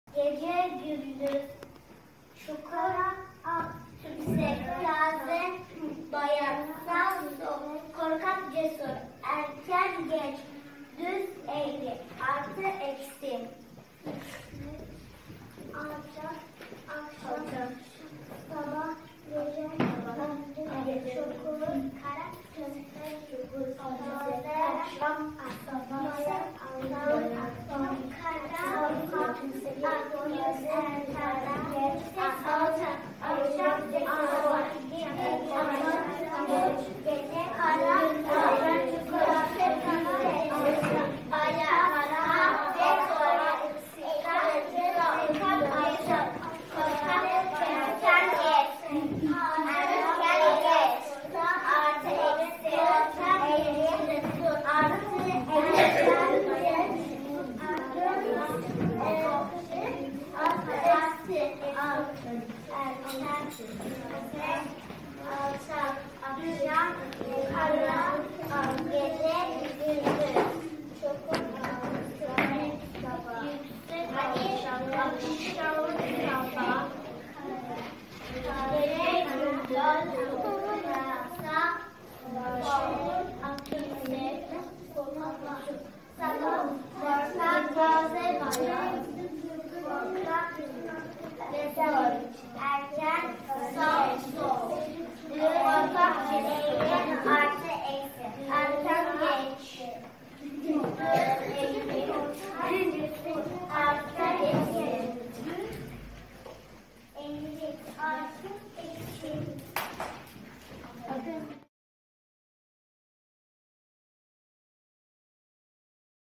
Zıt Anlamlı Kelimeler 1.Sınıf Balıklıdere İlkokulu
Zıt Anlamlı Kelimelere Örnekler,Sınıf Etkinlikleri 1.Sınıf
İlkokul Etkinlikleri-Oyunlarla Öğreniyorum
Sınıf Etkinliklerimiz-Ebru RENDE